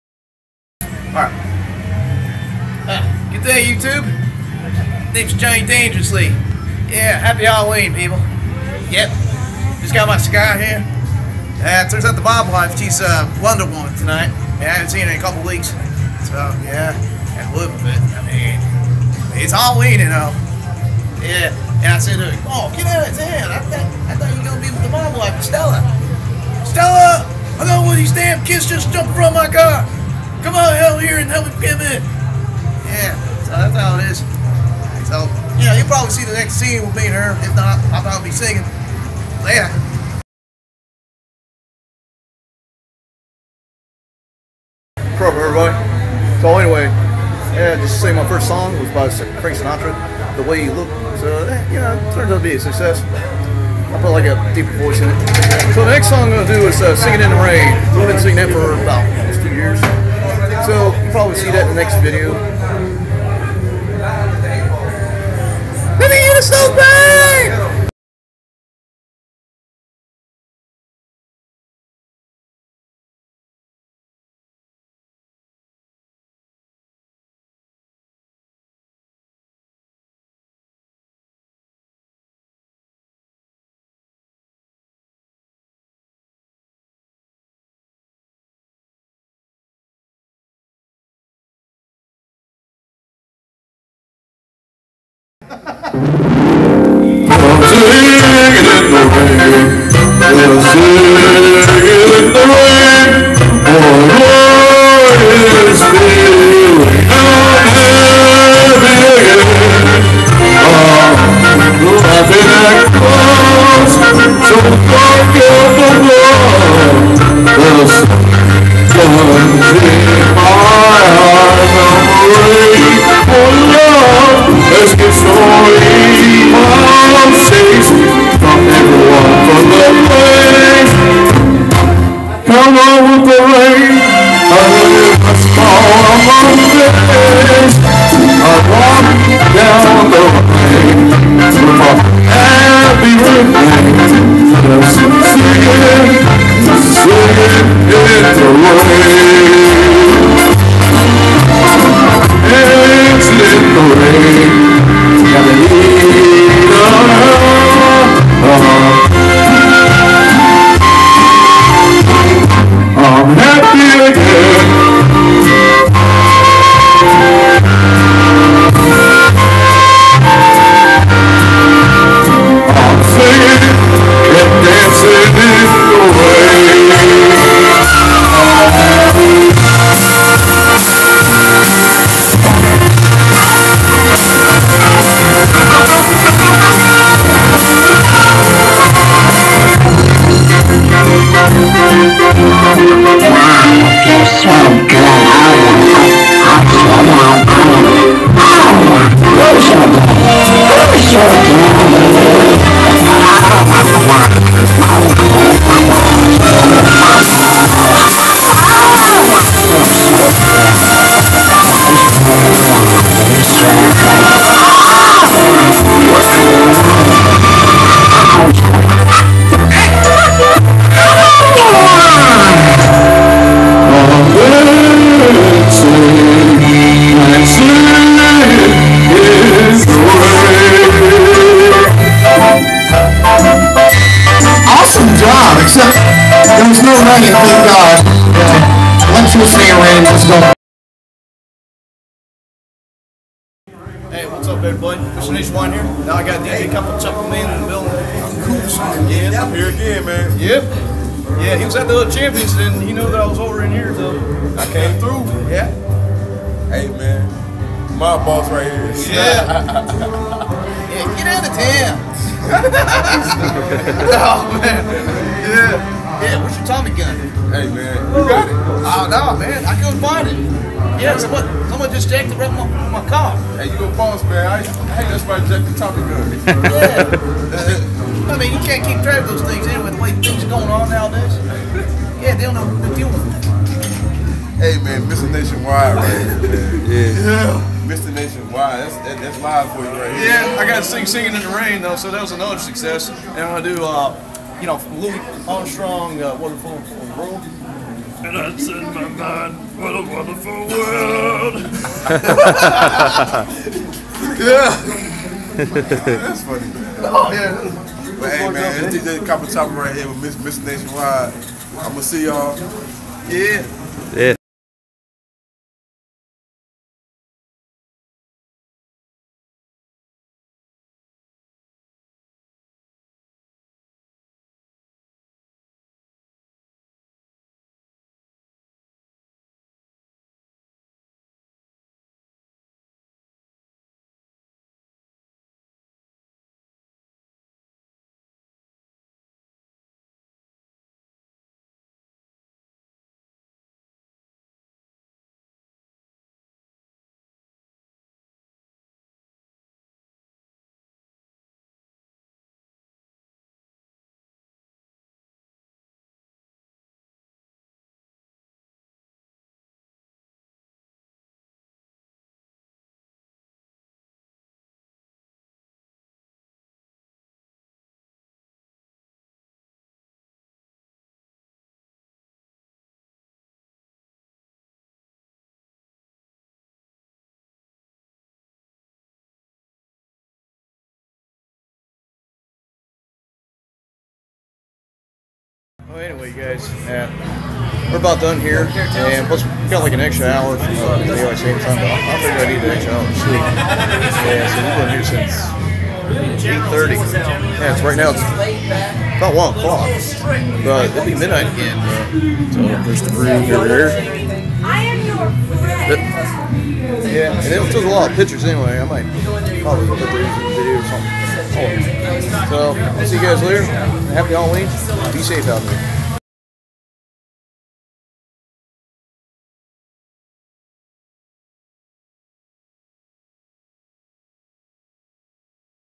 Halloween night at Champions
Having fun on Halloween night at Champions.
I was the Mob Boss.